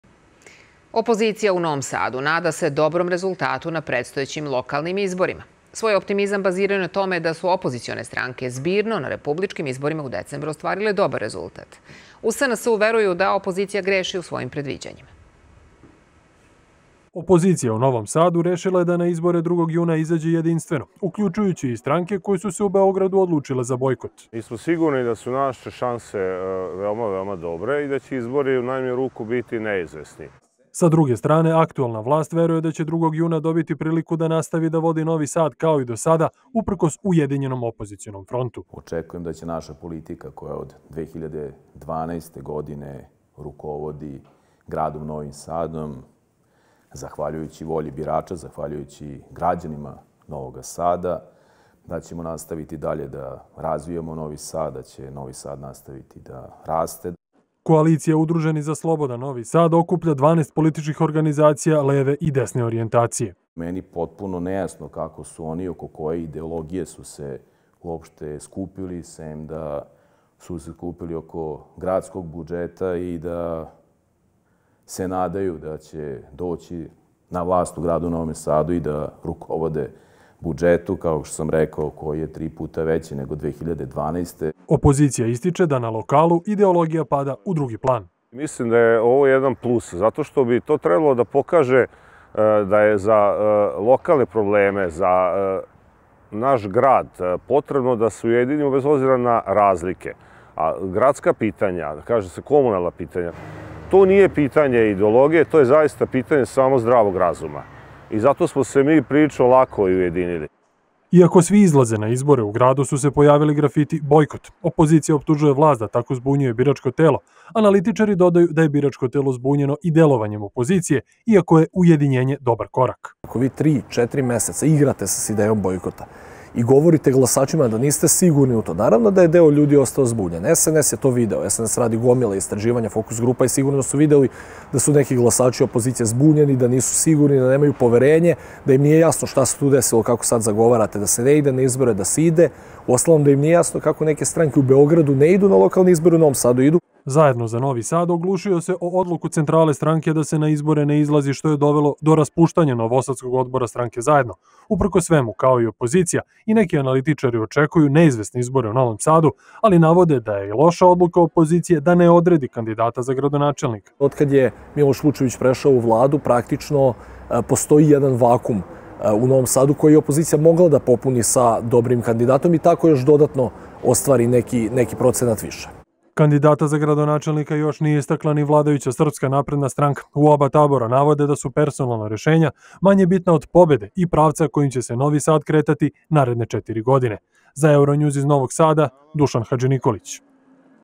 Opozicija u Novom Sadu ujedinjeno izlazi na izbore
Opozicija u Novom Sadu, nada se dobrom rezultatu na predstojećim lokalnim izborima u tom gradu. Svoj optimizam baziraju na tome da su opozicione stranke zbirno na republičkim izborima u decembru ostvarile dobar rezultat. U SNSu, veruju da opozicija greši u svojim predviđanjima.